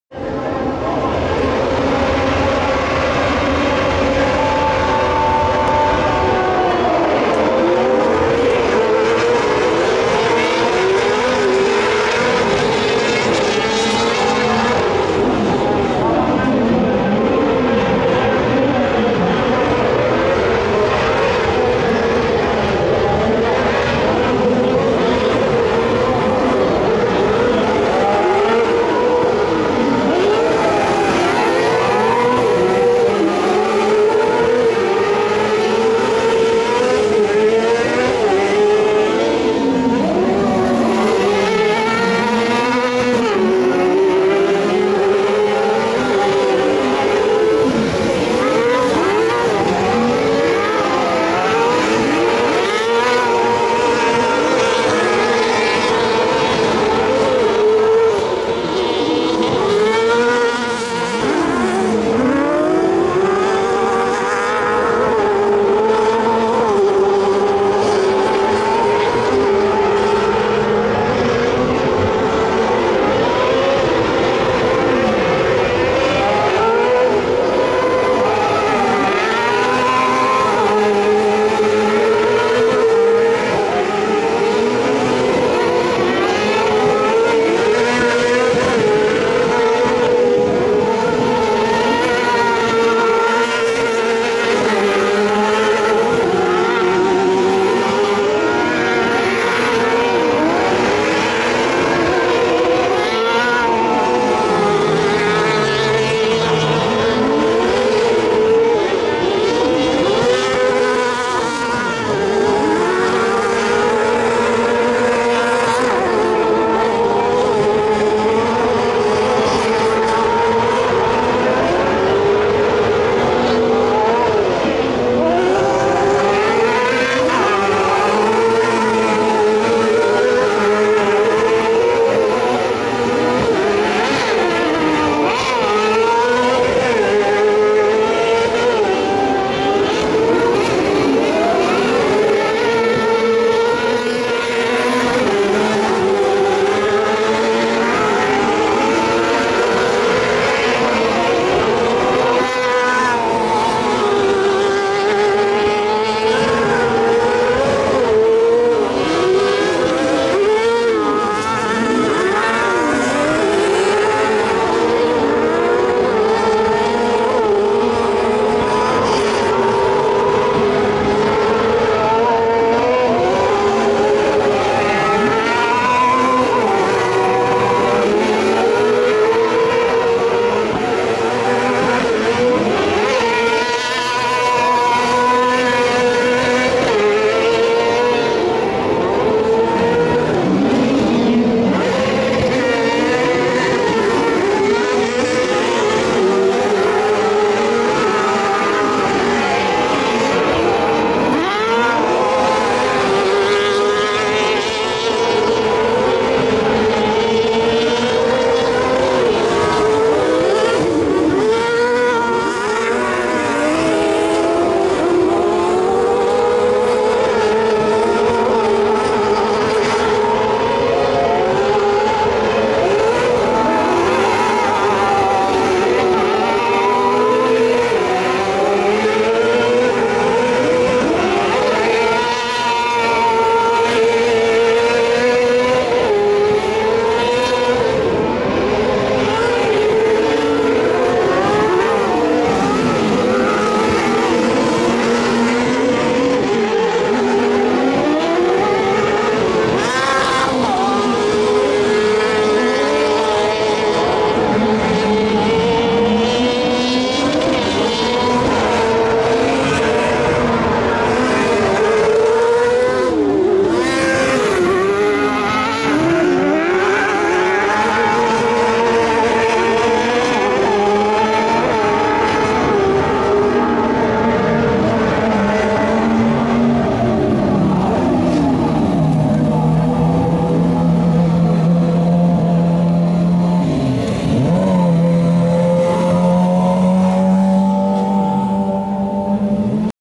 maggiora 2015 - junior buggy - heat 1 - group 1
Video recorded at the European Championship Autocross 2015, Race 8: Maggiora